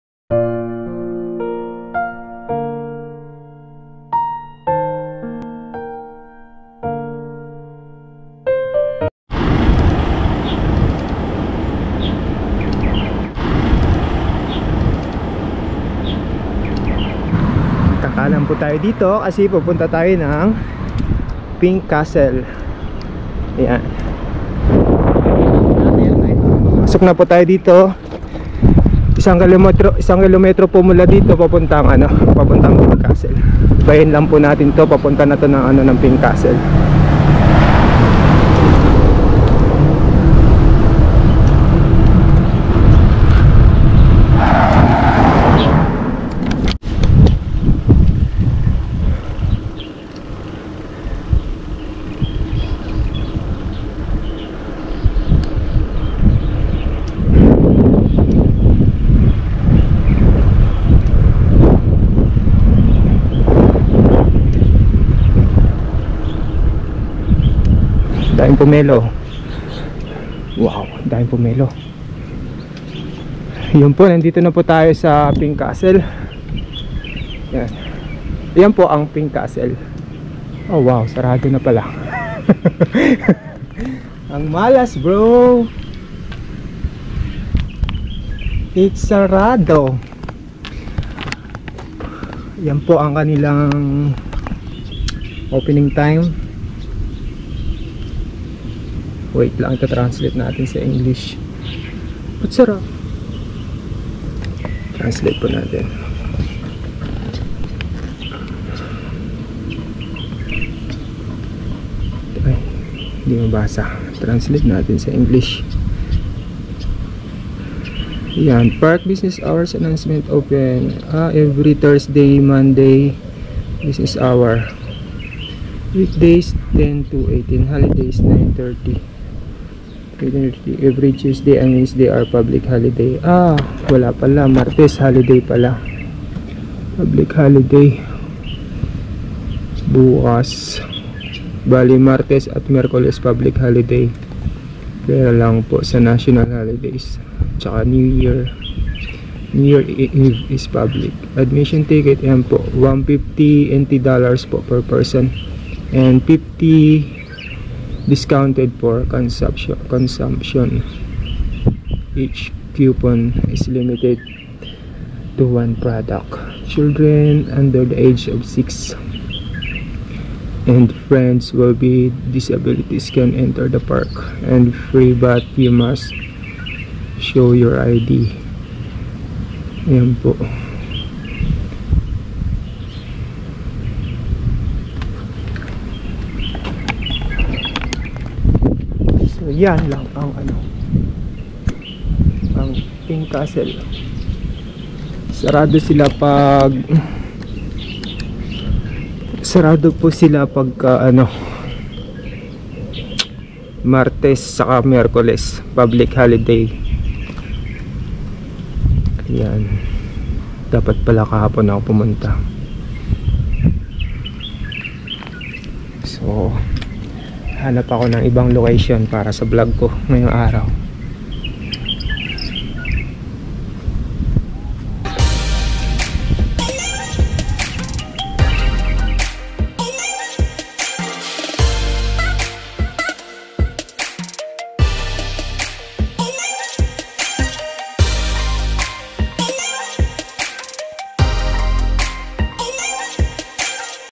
Pink Castle
646, Yunlin County, Gukeng Township, 湳仔50號
Park business hours announcement
The park opens every Thursday and Monday, and the business hours are
Weekdays 10:00~18:00, holidays 9:30~18:30.
2. Every Tuesday and Wednesday are public holidays (except for national holidays), and New Year's Eve is public holiday.
3. The admission ticket is 150 yuan per person, and 50 yuan can be used to offset consumption.
4. Children under the age of six and friends with disabilities can enter the park for free,
But you must show your ID.
5. Pets can be brought into the park, but the owner should be responsible for safety and hygiene
Of responsibility.
6. There is no smoking in this park, and no outside food is allowed.
Please cooperate.
March 1, 2019 Announcement